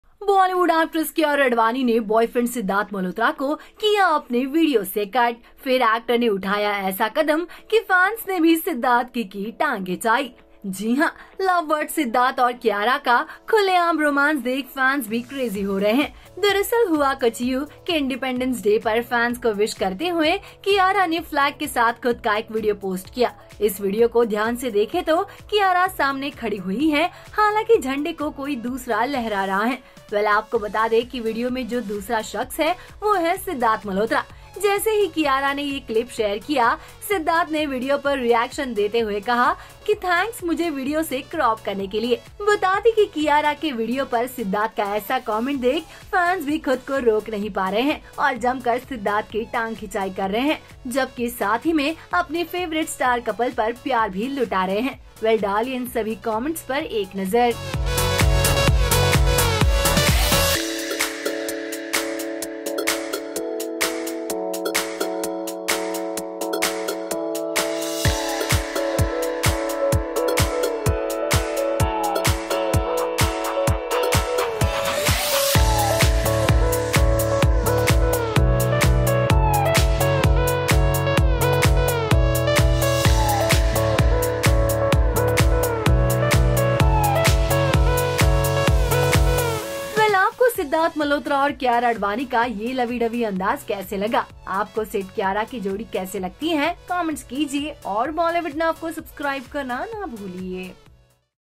Cute Couple Fights! Sidharth Malhotra Upset With Kiara Cropping Him From Independence day Video 😞
On India's 76th Independence Day, Kiara shared a video of her with the national flag. As soon as Kiara shared the clip, Sidharth reacted by saying that she cropped him from the video. Have a look